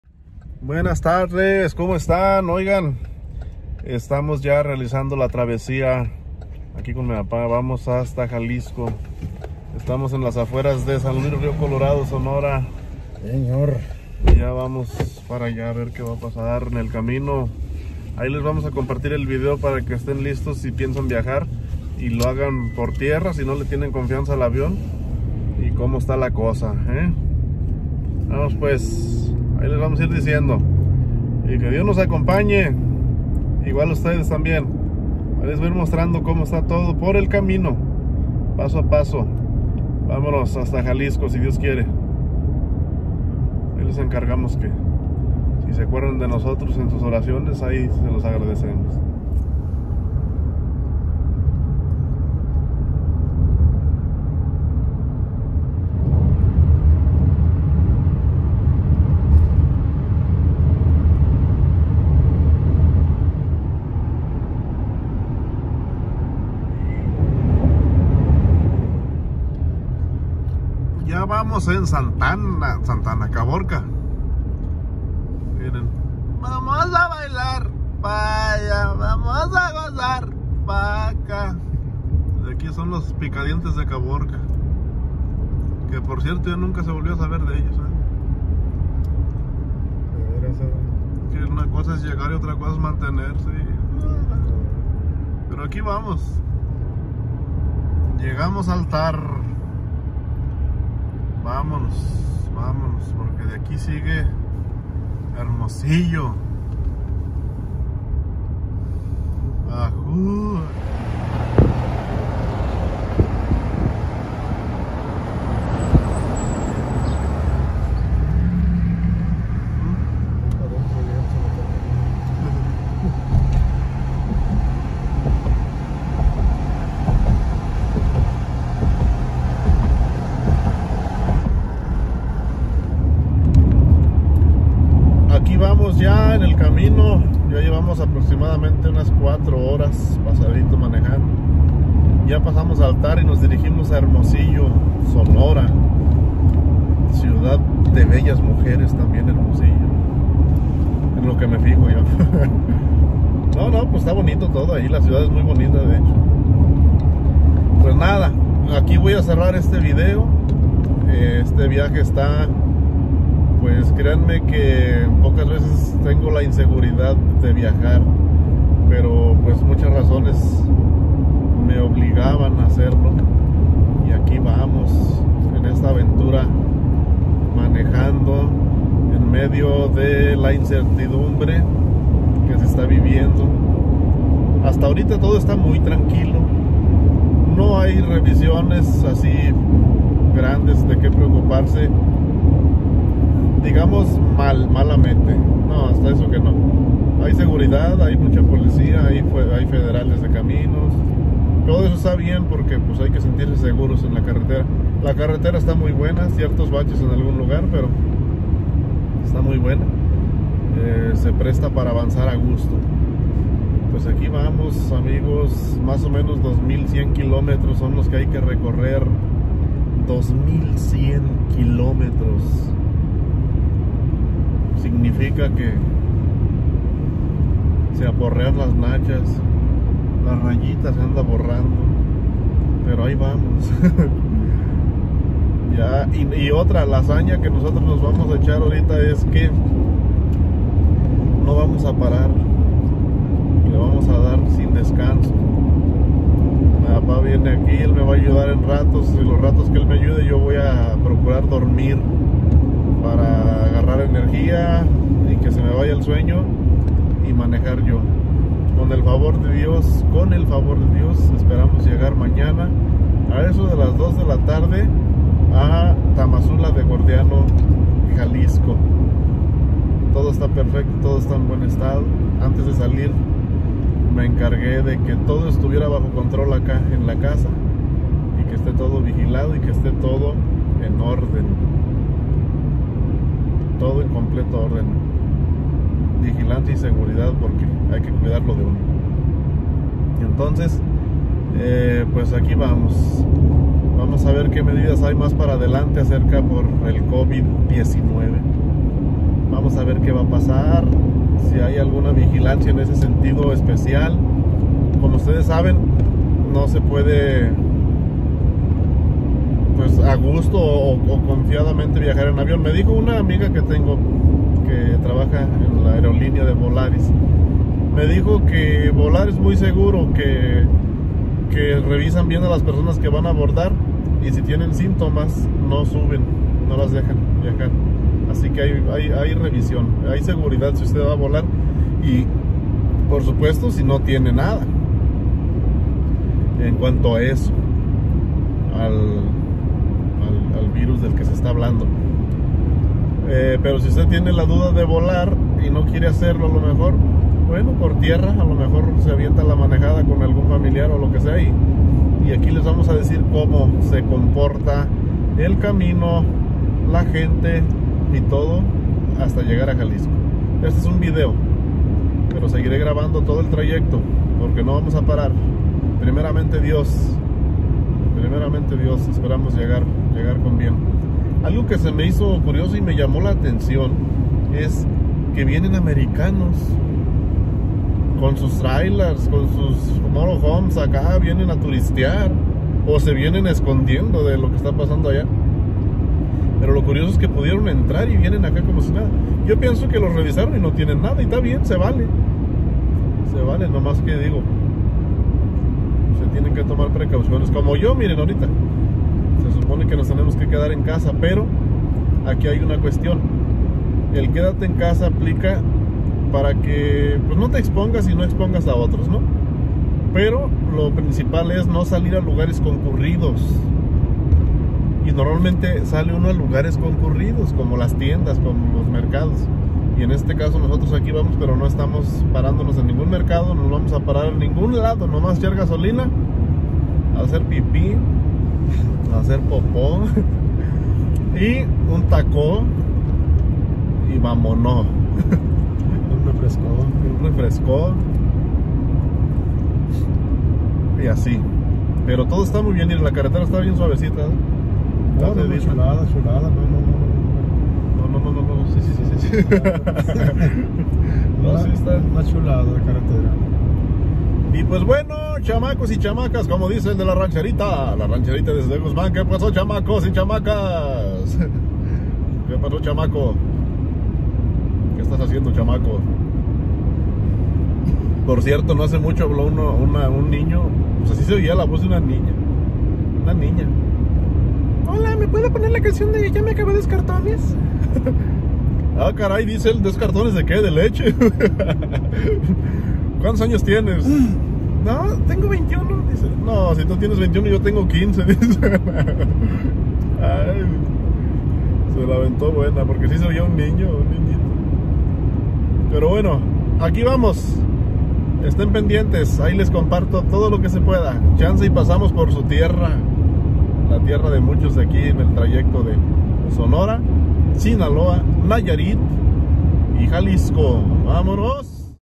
Viaje Arriezgado Mexicali - Guadalajara Manejando Pte 1
Manejando desde Mexicali hasta Guadalajara Jalisco con muchos riesgos pero aquí vamos.